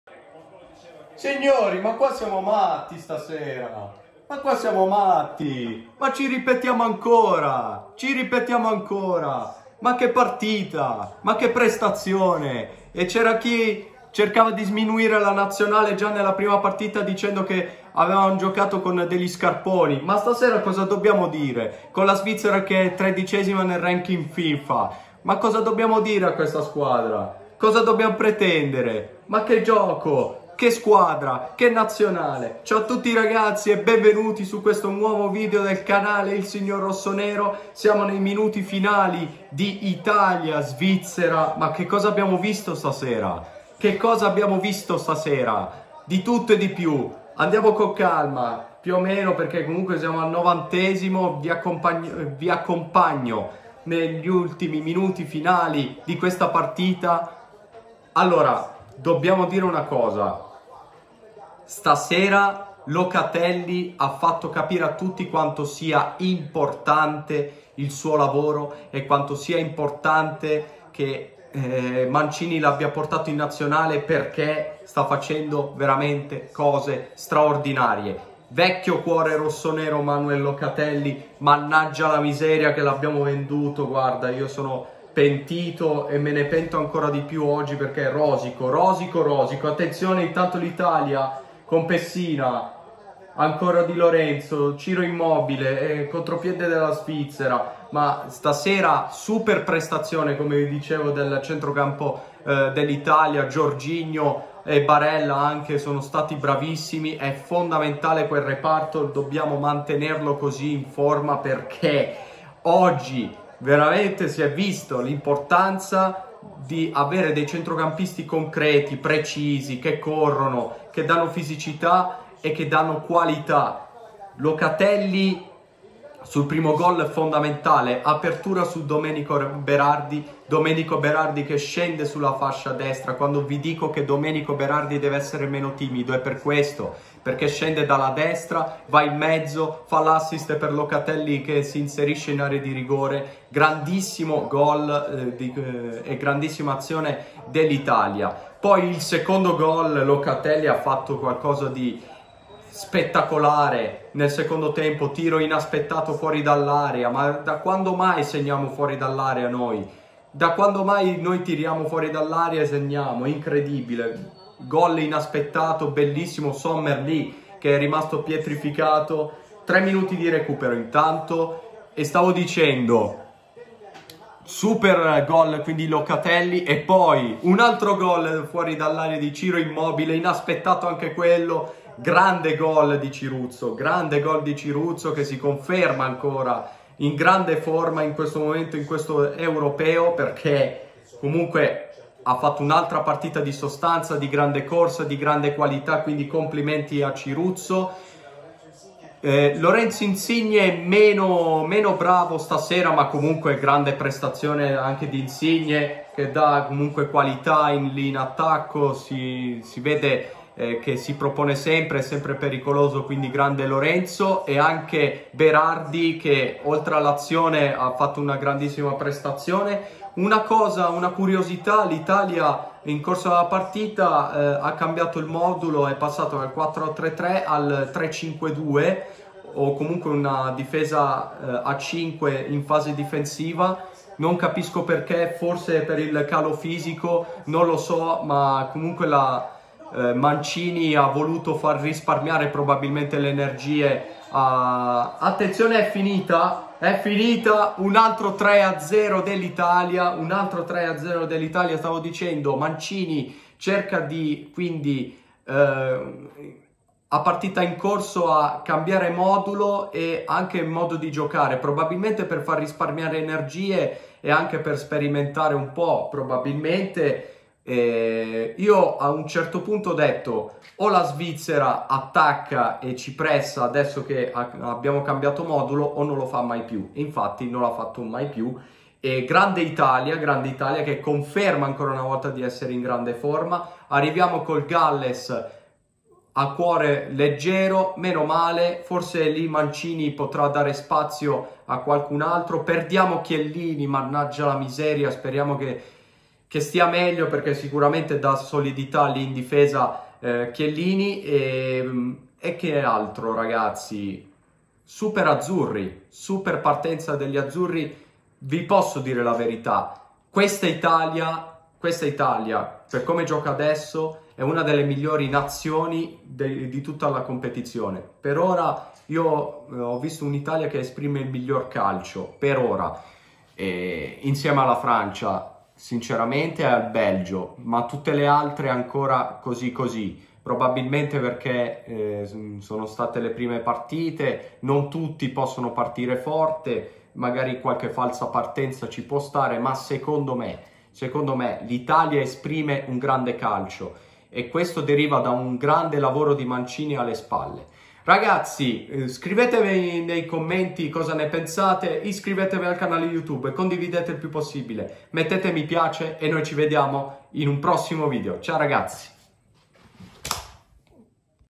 ITALIA- SVIZZERA 3-0// E SI GODEEEEE SIAMO AGLI OTTAVIII, GRANDE LOCATELLI!!!
Ed ecco la seconda vittoria dell’Italia che ci permette di passare il prossimo turno!!! Grandi ragazzi sono orgoglioso di voi!

✅COMMENTO

Popolo Rossonero mi segua su Instagram🔴⚫️ : il_signor_rossonero